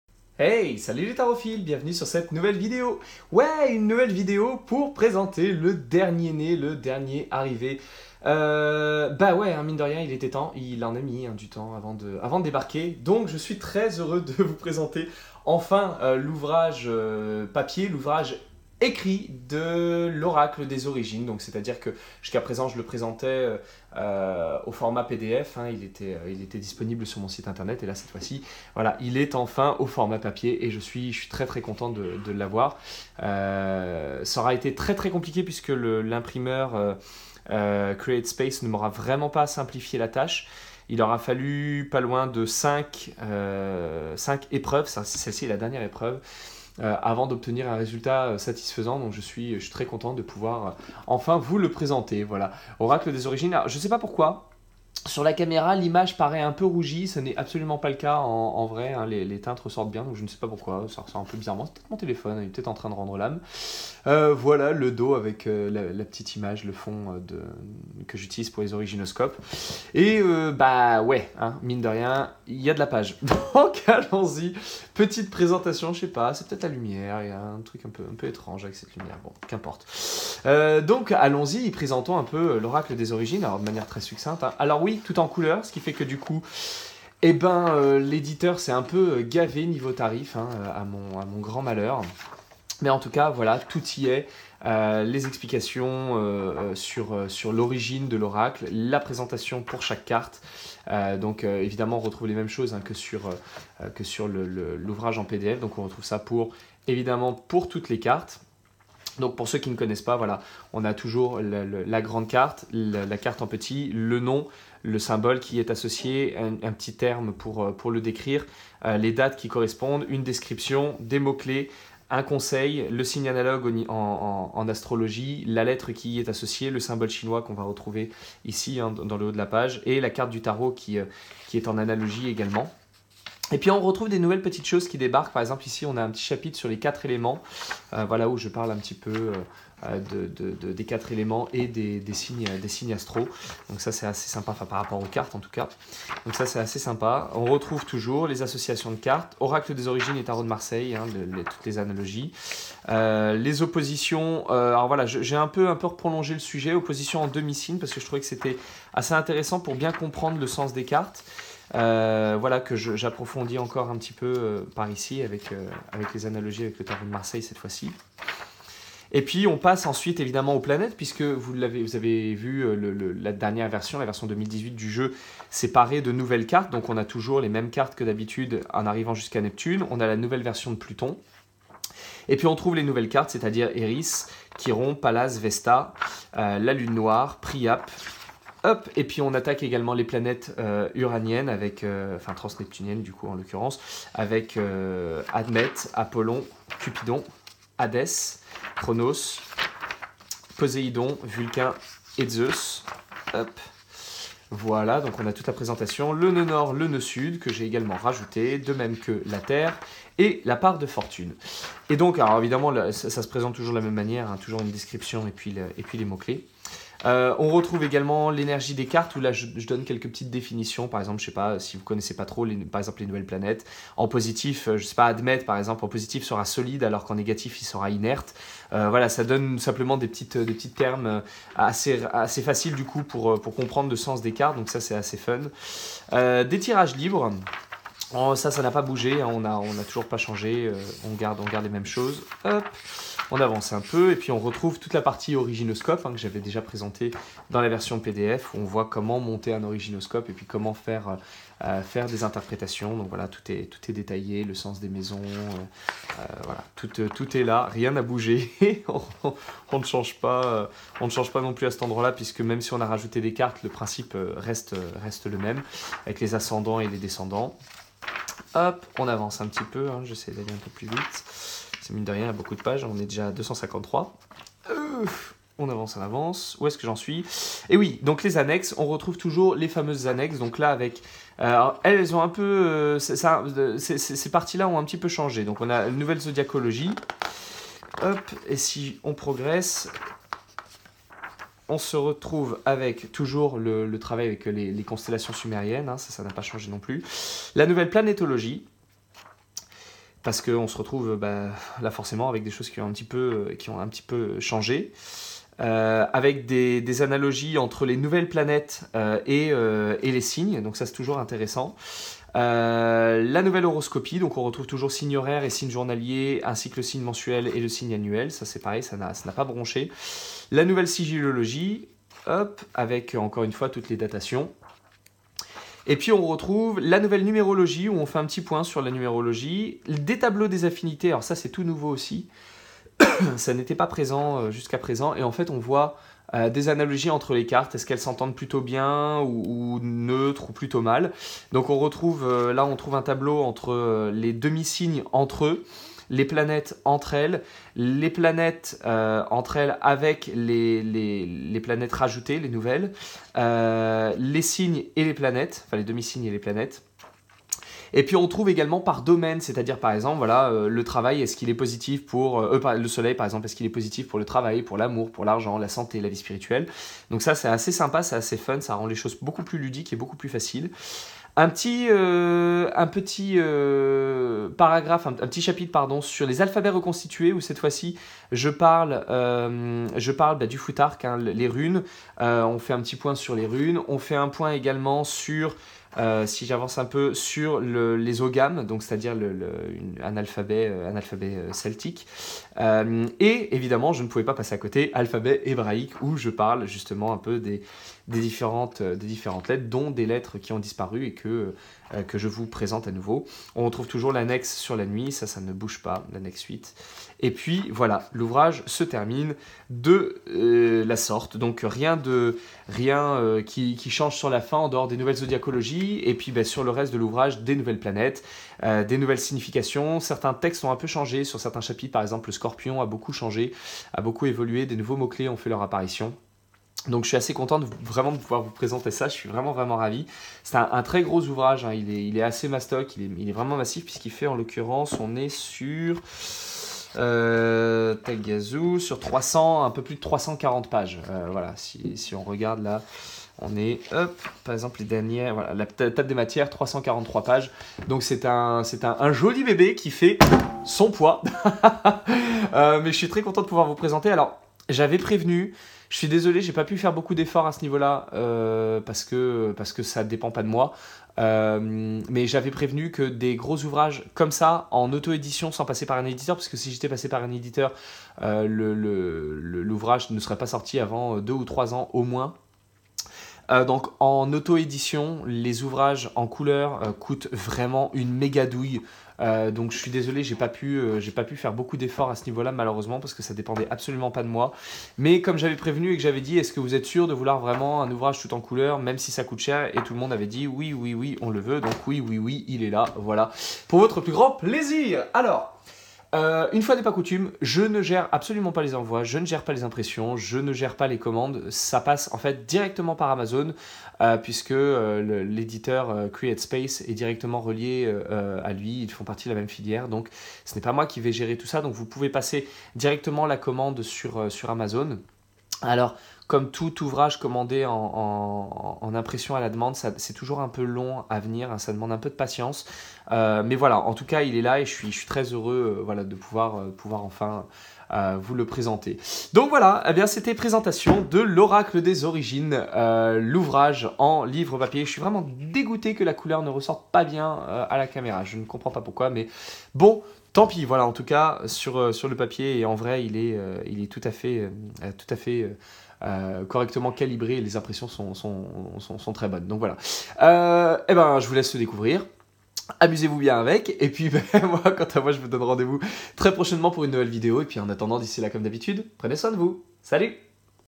📚 [LIVRE] L'Oracle des Origines
Présentation de l'ouvrage papier de l'Oracle des Origines, imprimé par les éditions CreateSpace et disponible via Amazon au lien suivant